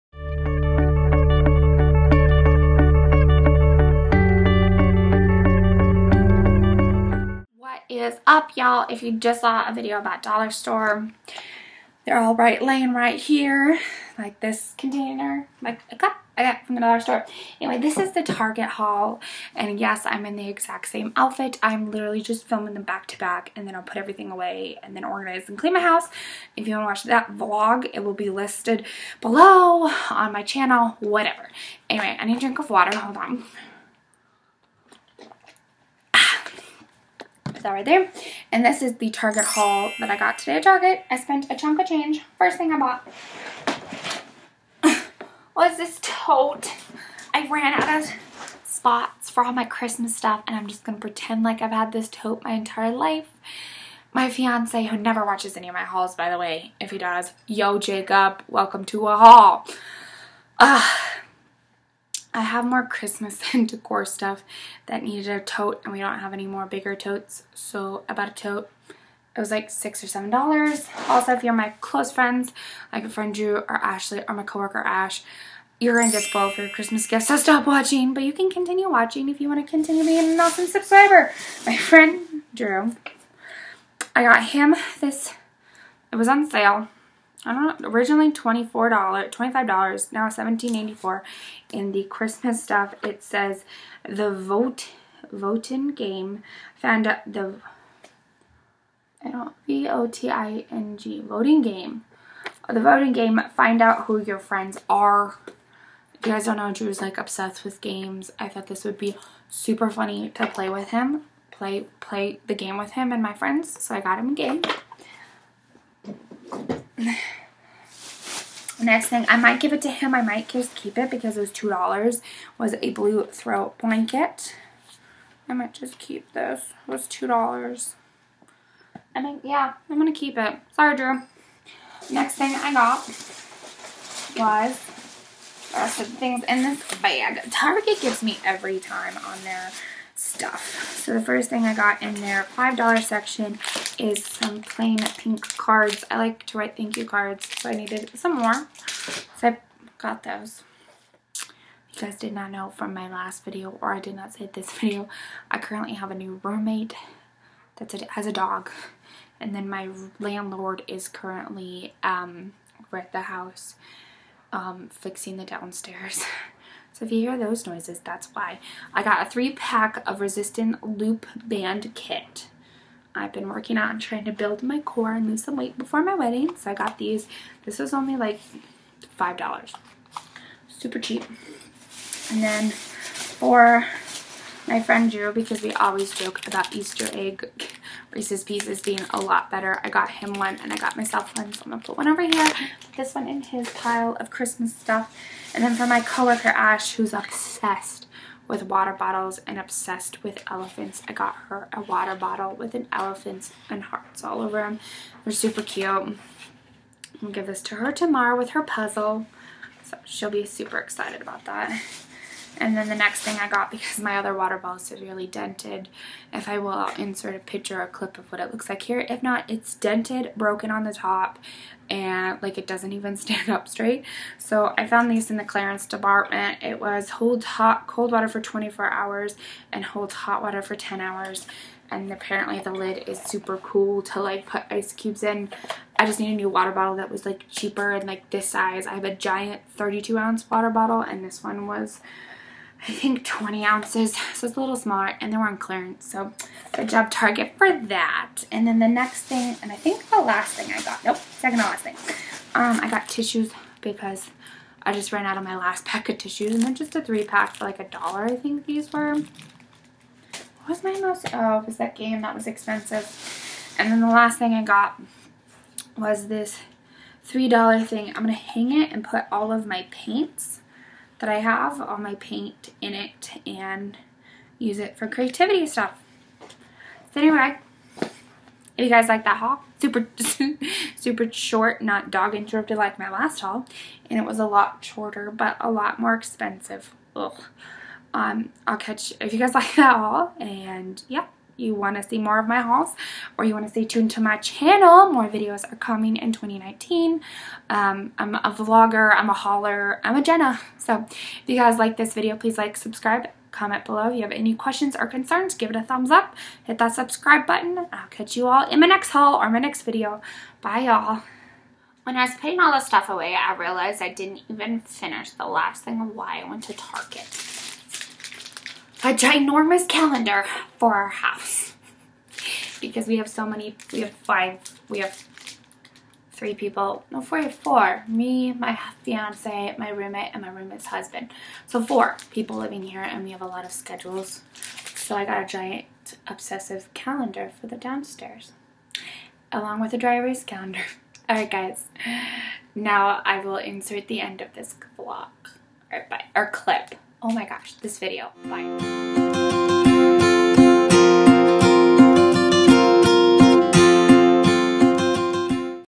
Target Haul 2019 | Jenna Osborn | Friends Gifts
I'm so sorry that this is so late going up. I hope that you love this video. Let me know if you love Target as much as me!! Have a good day!!- Jenna

Follow me on !
                    -@Jennabooklover (book one)
- @butterflycutie94 (Model)
- jennaophoto_video ( Photo page)
Twiter:  @jennaosborn123
Goodreads: Jenna Osborn